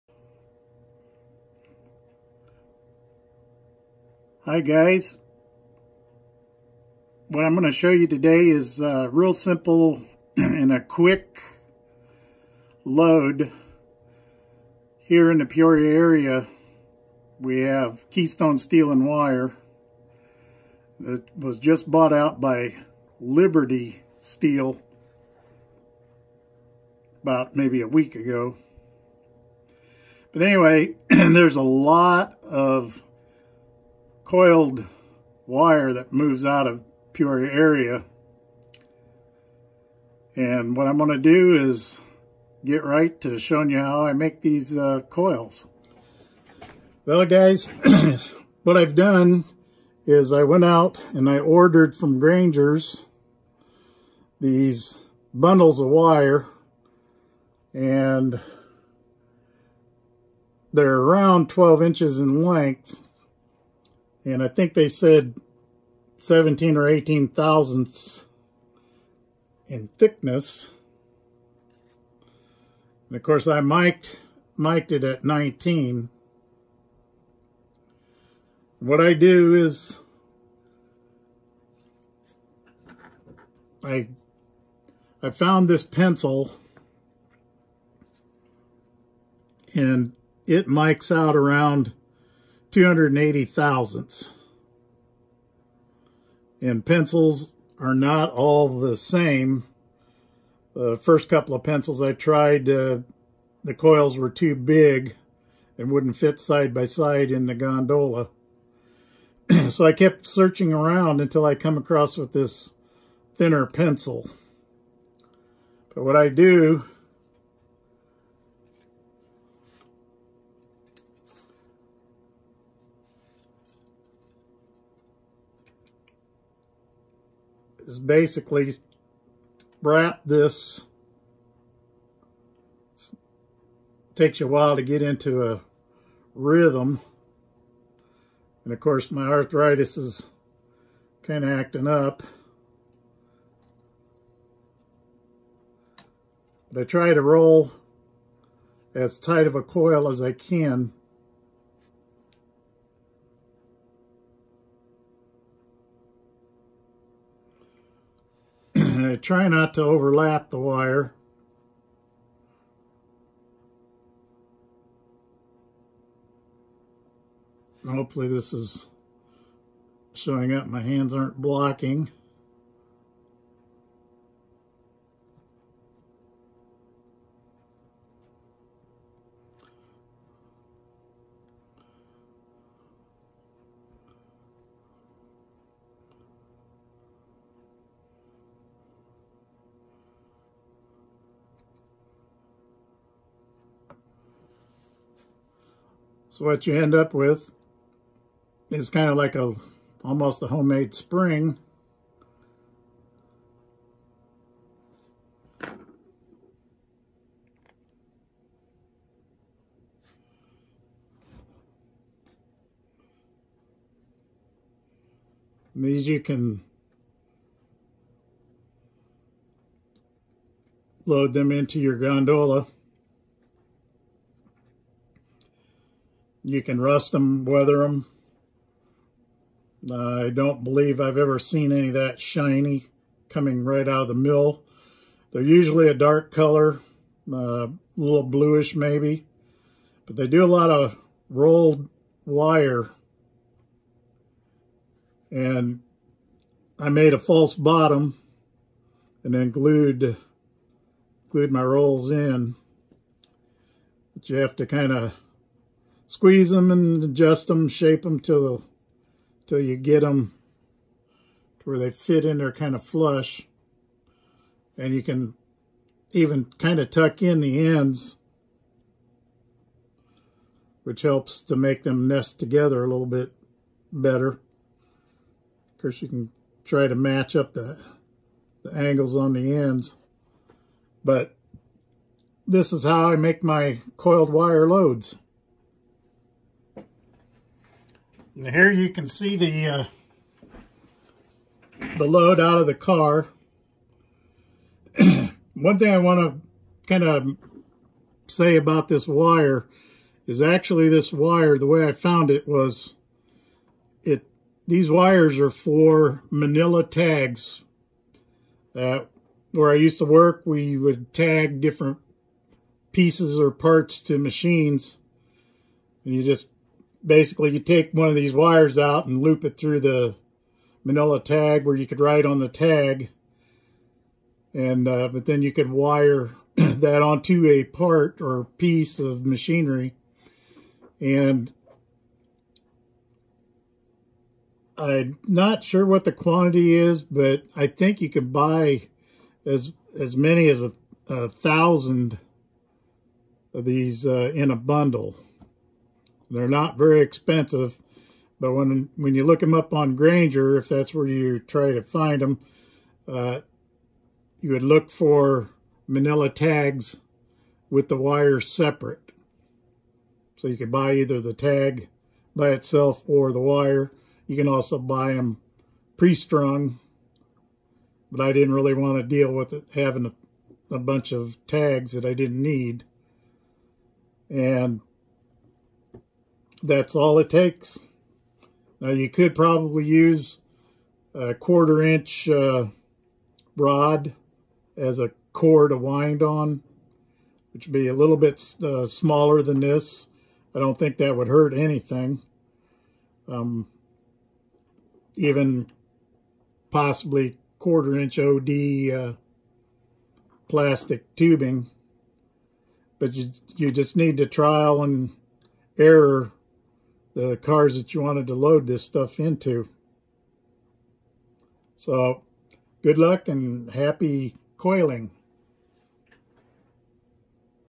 GL&S #16 N Scale coiled wire open load
GL&S Making an N Scale coiled wire open load from .019” tag wire.